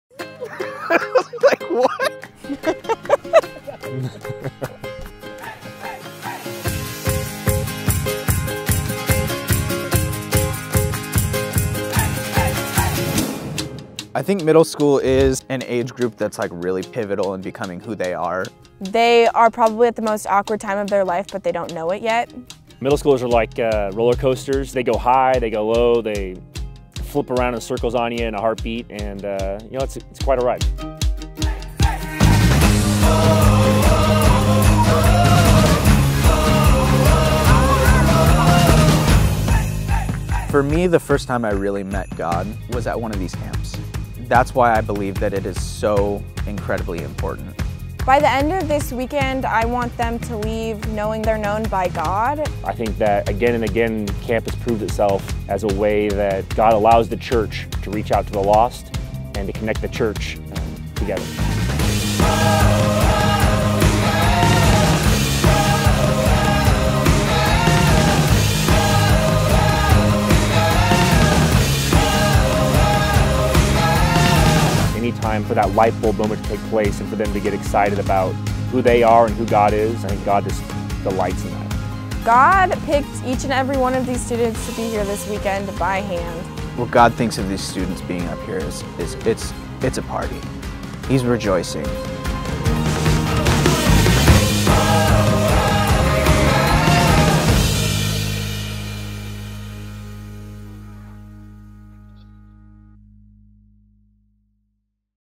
Triumph Winter Camp - 2020
LCC's Middle School Ministry (Triumph) Winter Camp at Mountain Meadows Bible Camp.